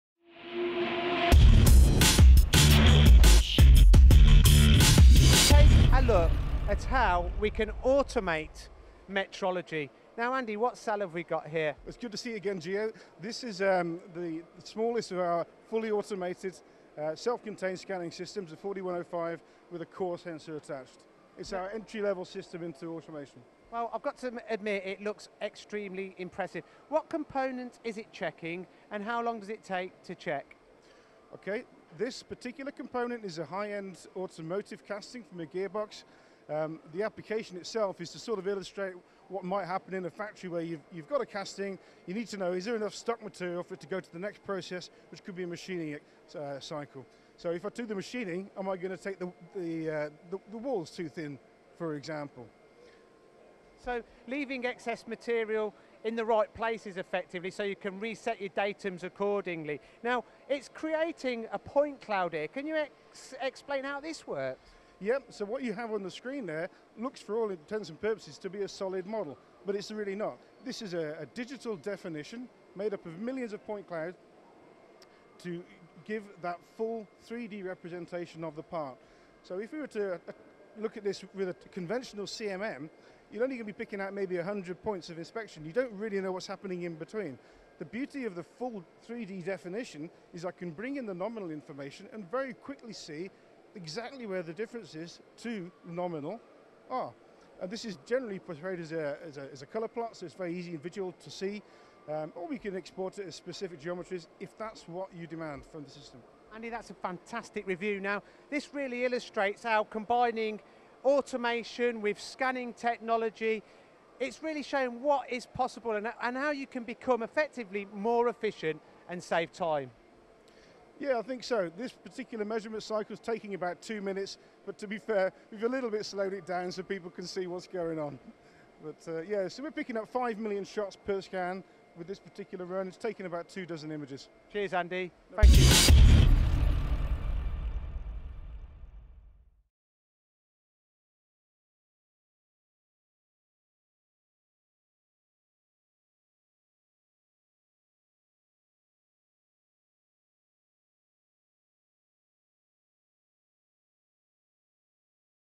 Measure tight tolerances scanning automatically
How do you try to take errors and reduce operator input out of measuring irregular parts, castings and other tricky components? how about this from GOM and FANUC.